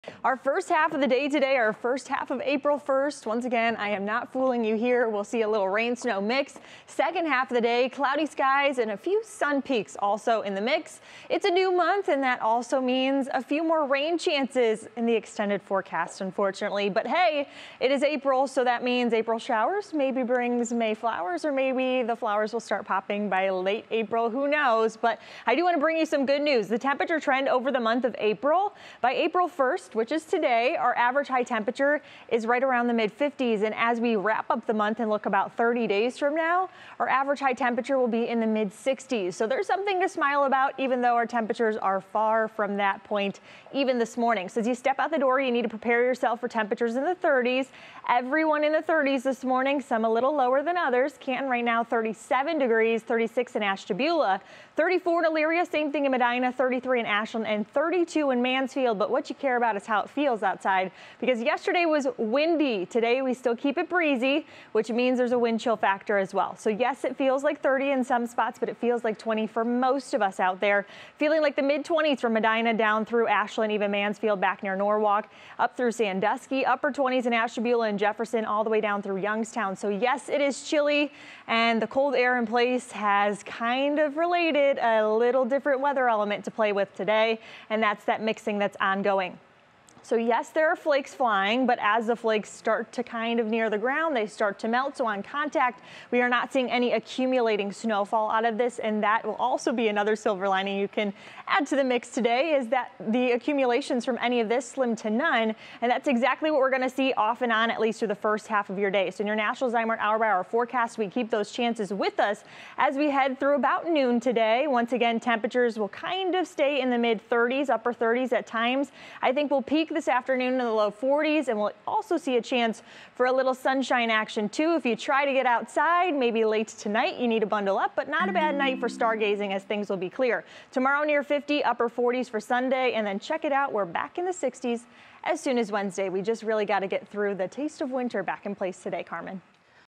Cold with snow: Cleveland weather forecast for April 1, 2022
It may be April Fools' Day, but this is no joke! We have colder temperatures and some snowflakes flying throughout Northeast Ohio. Your Cleveland weather forecast shows scattered snow into the afternoon before conditions quiet down. Highs today will climb to around 40 degrees. Saturday will warm to around 50 with some rain late into Sunday. Looking ahead, temperatures will warm to the upper 50s by next Tuesday with the mid 60s expected by Wednesday.

3News' Payton Domschke has the hour-by-hour details in her morning weather forecast for Friday, April 1, 2022. See the extended weather forecast